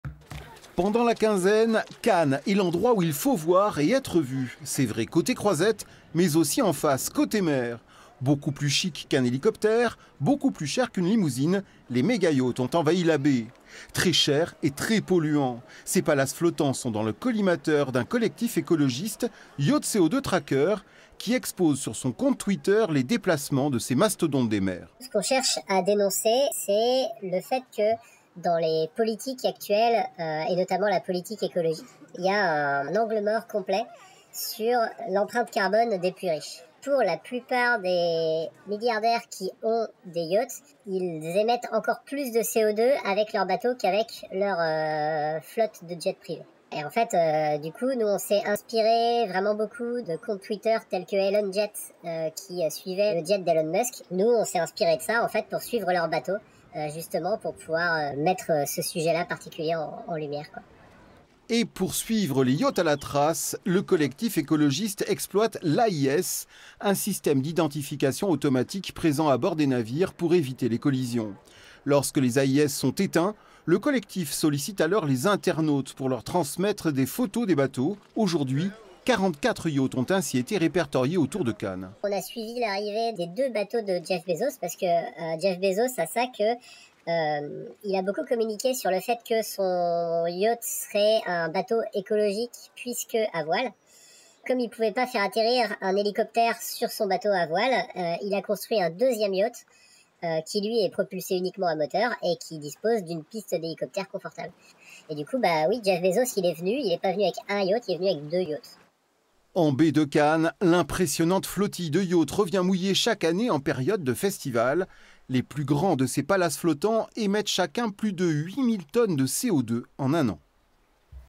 Yacht CO₂ tracker, ce collectif veut "traquer" les yachts
Le strass et les paillettes de Cannes pourraient bien laisser la place à une autre exposition moins glamour. Un collectif se propose de montrer du doigt les yachts les plus polluants. Une sorte de baromètre de CO₂ émis par de riches propriétaires de bateaux. Petites révélations plutôt embarrassantes.